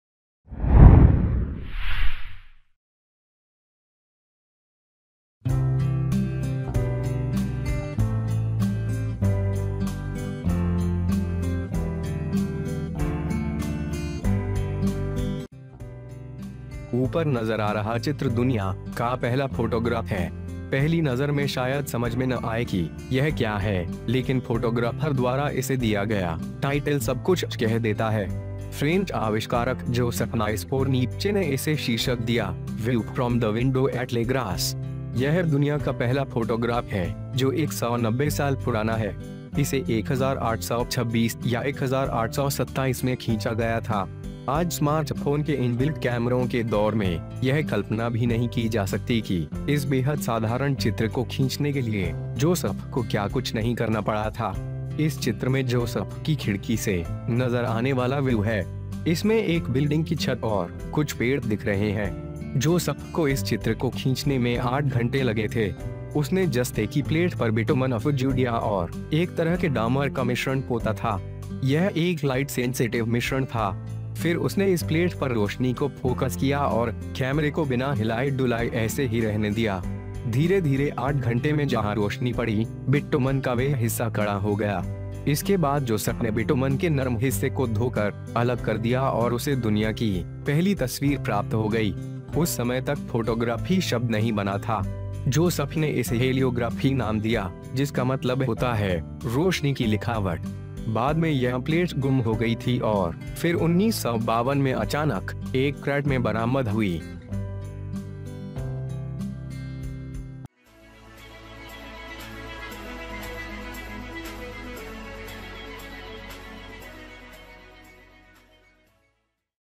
ये है दुनिया की पहली फोटो, क्या आपको समझ में आया कि ये क्या है?
The picture seen above is the world's first photograph. At first glance, it may not be possible to understand what this is, but the title given by the photographer gives everything. The French inventor Joseph Nisför Niepche gave the title - 'View from the window at le grass'. This is the world's first photograph, which is 190 years old. It was drawn in 1826 or 1827. Today, in the era of smartphone inbuilt cameras, it can not be imagined that Joseph did not have to do anything to draw this very simple picture

In this picture there is a view from Joseph's window. There is a building roof and some trees are visible in it.
-Joseph took eight hours to draw this picture. He used to mix Bitumen of Juda and a kind of asphalt on the zinc plate. It was a light sensitive mix.
Then he focused on the light on this plate and let the camera move like this without any hesitation. Slowly, in 8 hours, where light grew, that part of bitumen became tight.
After this, Joseph washed off the soft part of bitumen and got the world's first picture.
Until that time the word photography was not created. Joseph named it a hieroglyph, which means - the handwriting of light.
Later this plate was lost and then in a crate suddenly recovered in 1952.

HJ NEWS !!